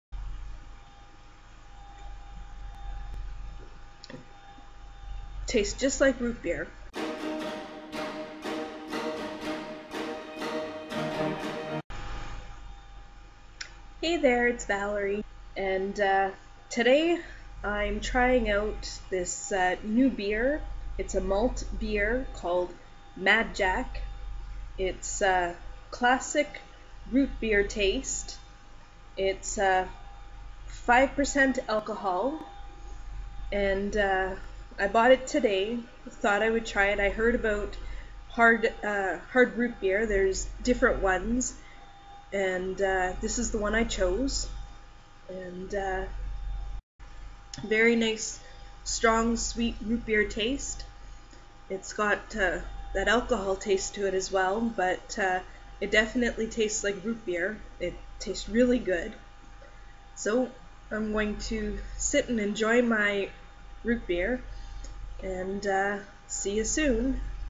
Hard RootBeer Taste Test!
Today i went to the Liquor Mart and Bought some hard rootbeer to try out!